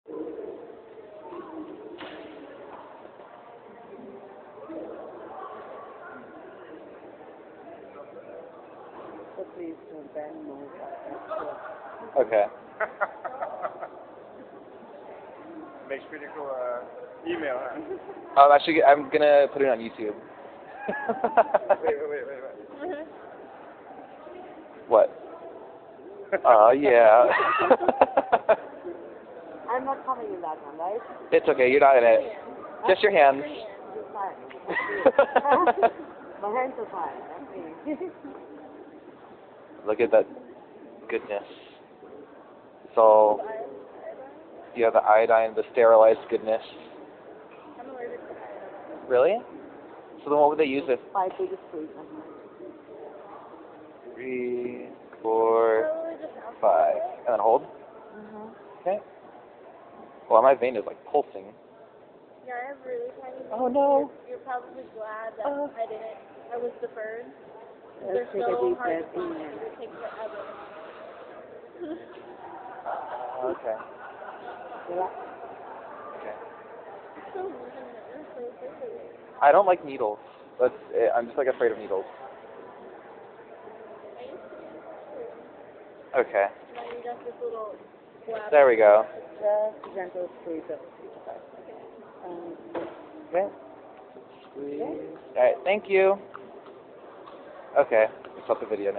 Feeding the vampires :D
I donated blood again this year for Music Saves Lives and decided that I wanted to film it. Sorry if seeing blood makes you a bit queezy. I actually had to look away while she was putting the needle in.
And sorry if the quality sucks. This was taken with my phone, so I don't expect it to be great.